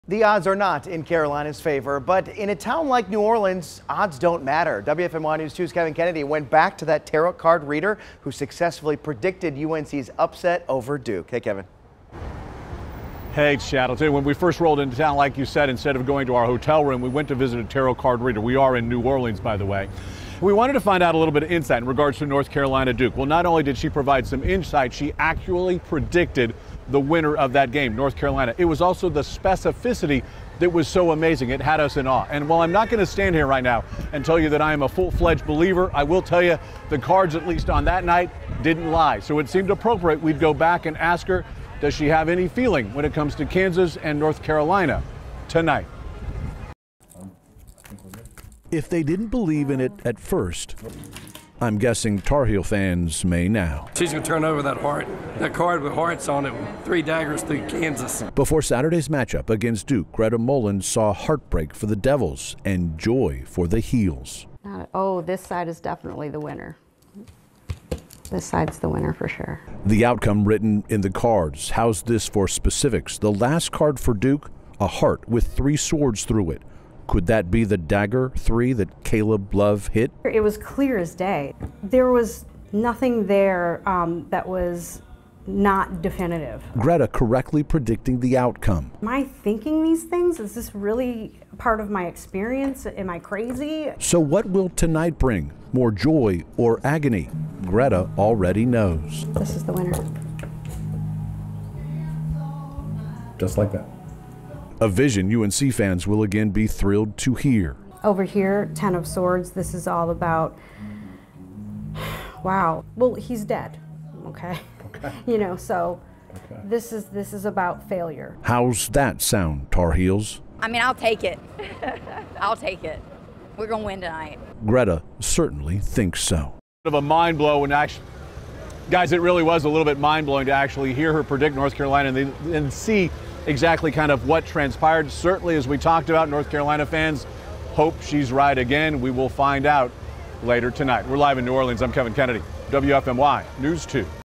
New Orleans tarot card reader predicts UNC win
WFMY News 2's Kevin Kennedy caught up with the tarot card reader from New Orleans who predicted a UNC win.

It’s a historic game but can it be predicted in the cards?

When in a city like New Orleans, rich with tradition, there’s a culture grounded in mystery.

For more than a dozen years, Greta Mellon has been unlocking questions, by seeing and feeling a spirit. “I think we all have the same gift,” Mellon said.